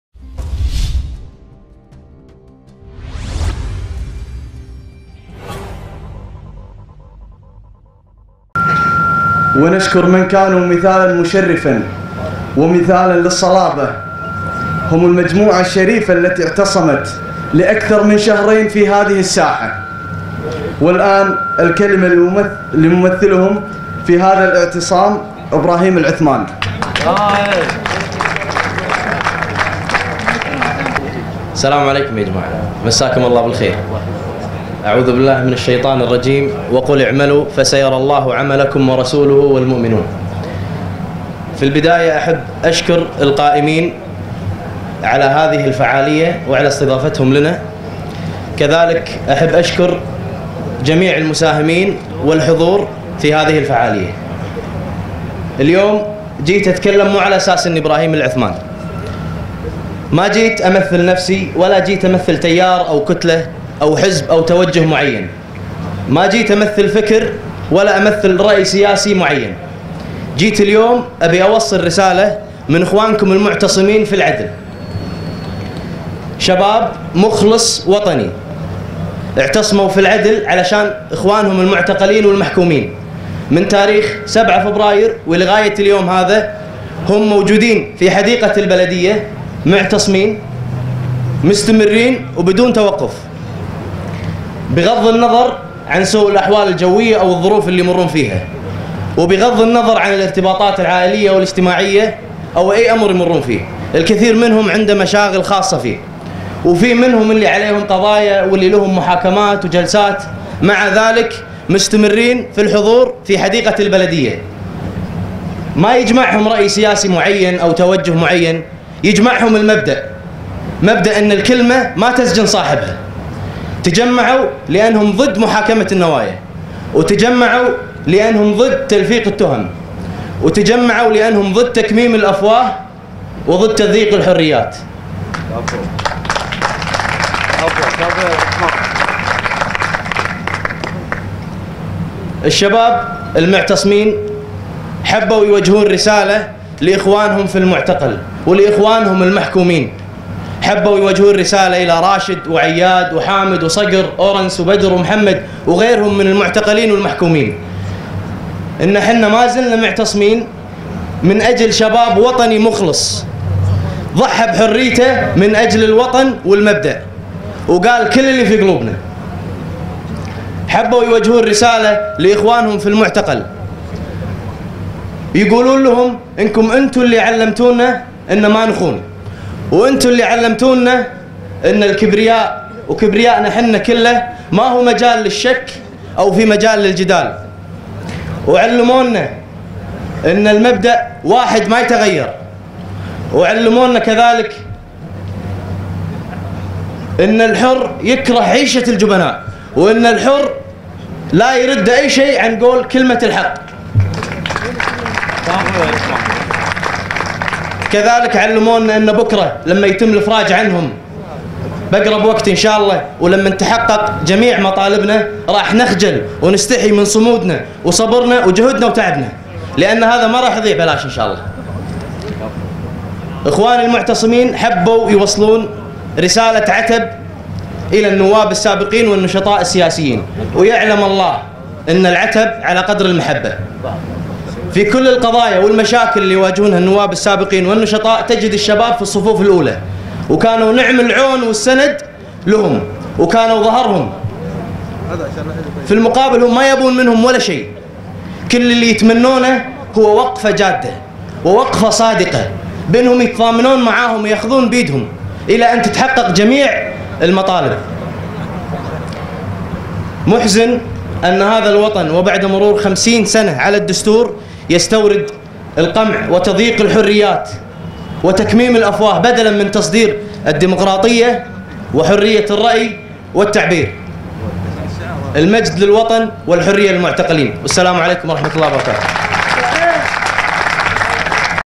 كلمة ممثل تجمع اعتصام العدل إبراهيم العثمان بأولى ندوات ائتلاف المعارضة من أمام قصر العدل 10-4-2013
فيديو: كلمة "إبراهيم العثمان" من أولى ندوات (ائتلاف المعارضة) من أمام قصر العدل بعنوان (الحكومة المنتخبة بمواجهة قمع الحريات) ..